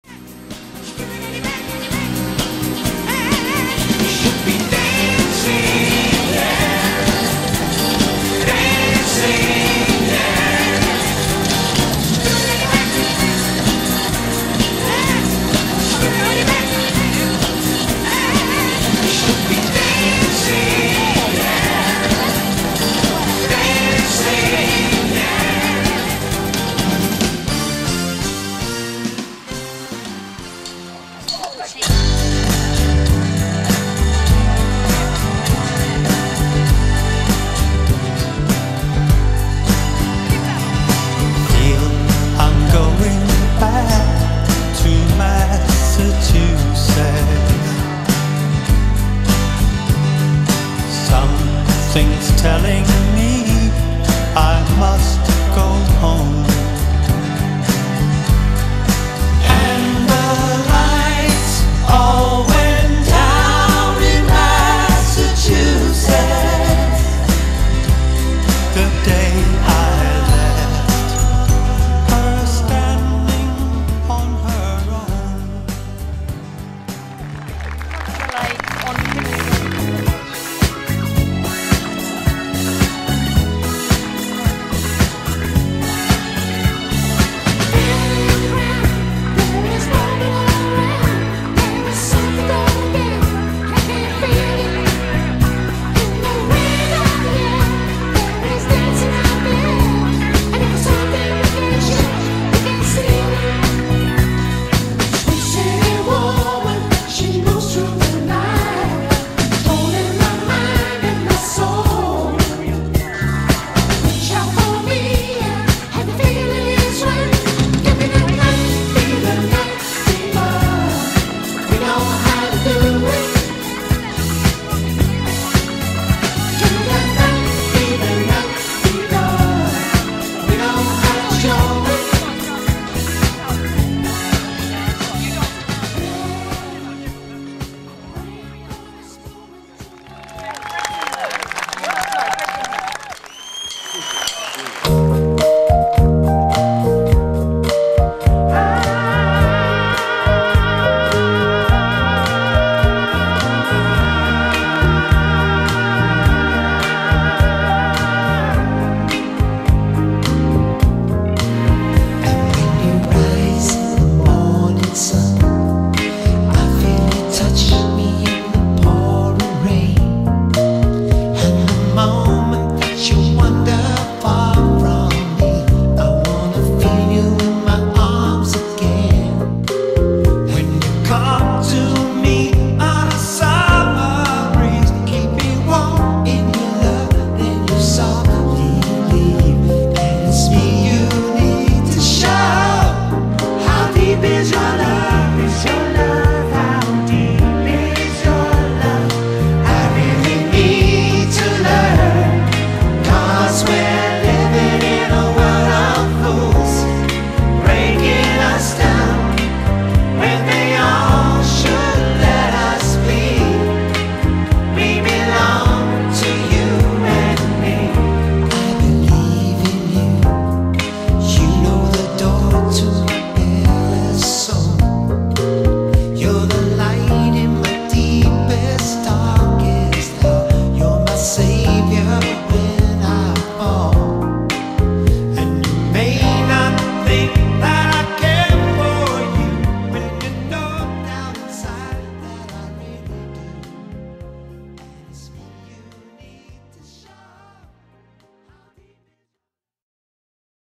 New Seekers Paul, Mick and Mark
professional and current bee gees tribute  'The Story of The Bee Gees'.